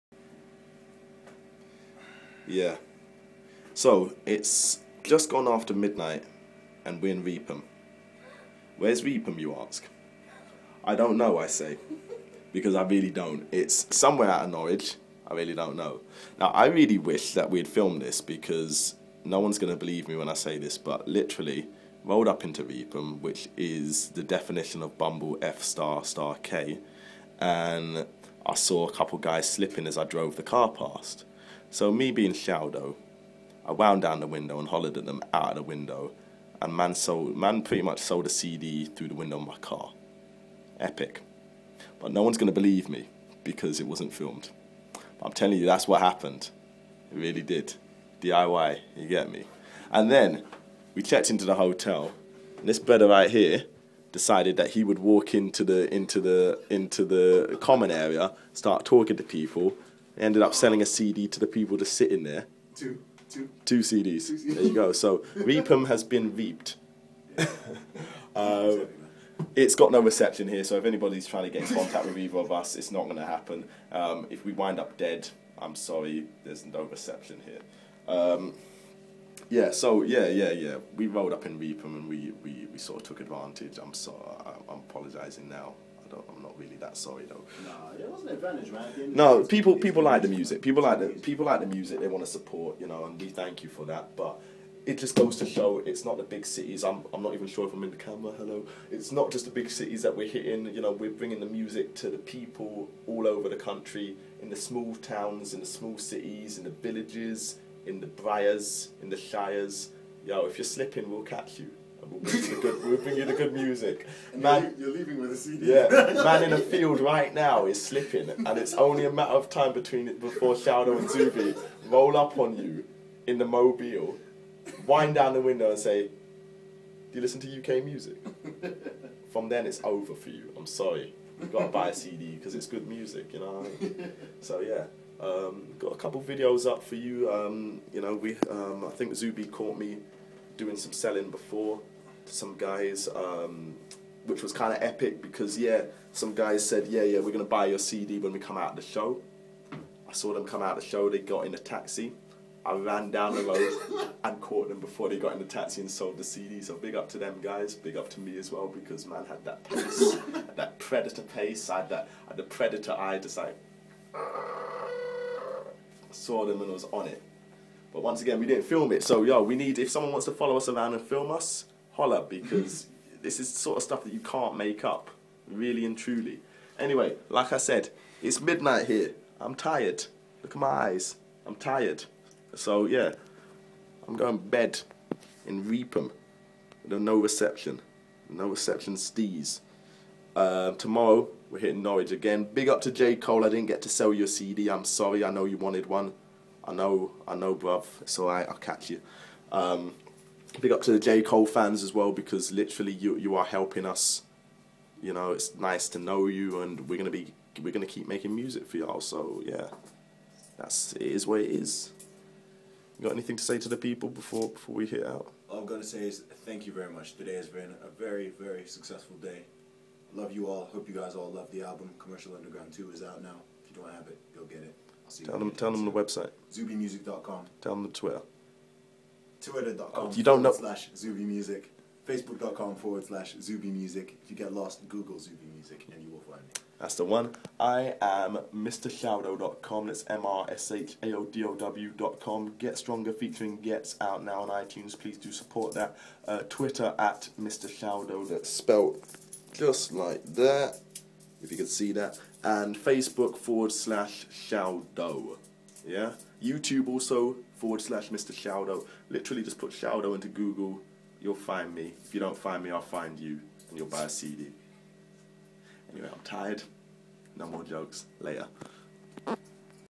ShaoDow and Zuby On The Road - Repping in Reepham
After a long day of shifting CDs in Norwich we made our way over to Reepham where the B&B was based.

On the way there I managed to sell a CD out of my car window and Zuby sold two in the hotel lobby.

Stop working? What does that mean??

DiY Gang
Team Zuby